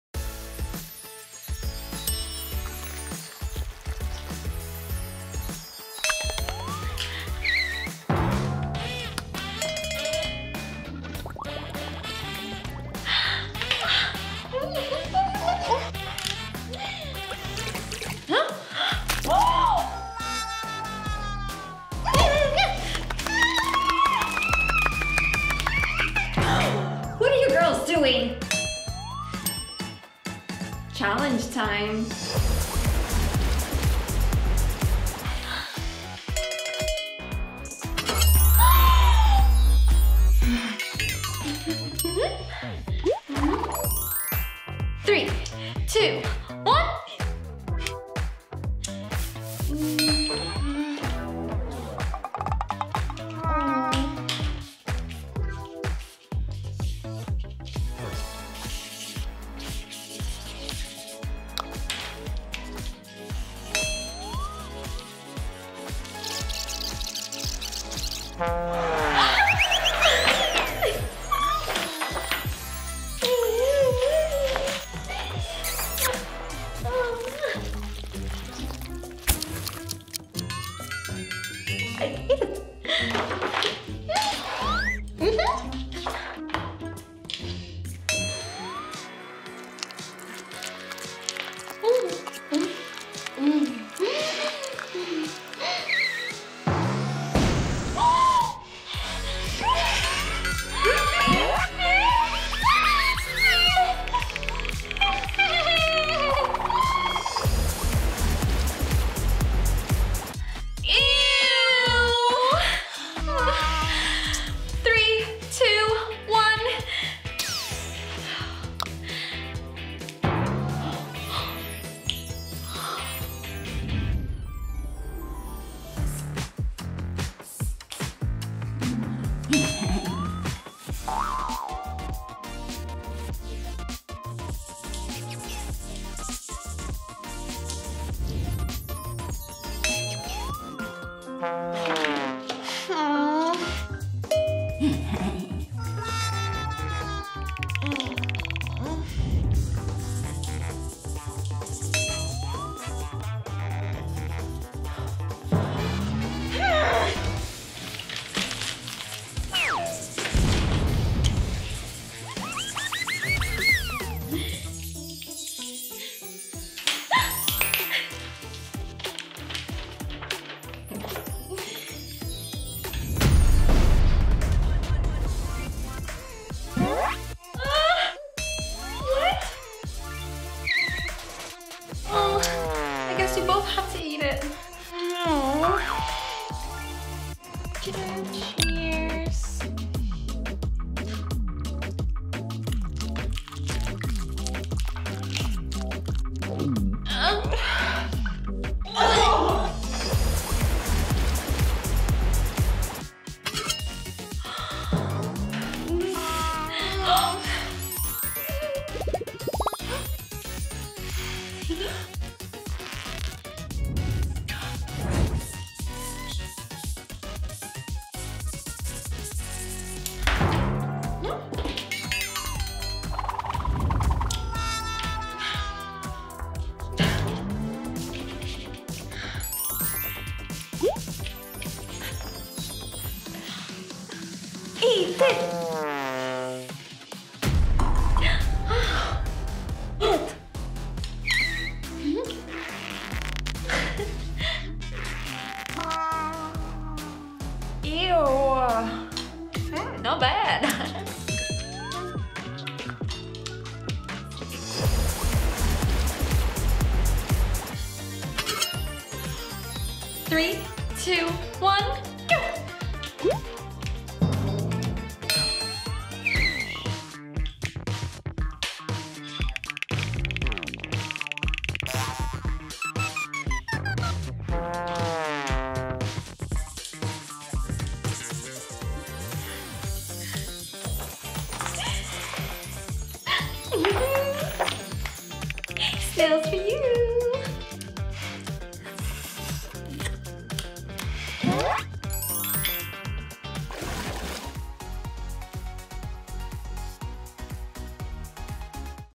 DRAW AND EAT FOOD CHALLENGE! Whoever Draws Better Will Eat It! A Fun School Food Challenge by BRAVO!
Well, who likes to stay in detention after school?  Of course nobody! What if there's an interesting food challenge? Sounds good already, doesn't it?
Check out the first part of the Draw And Eat Challenge with Summer and Stella!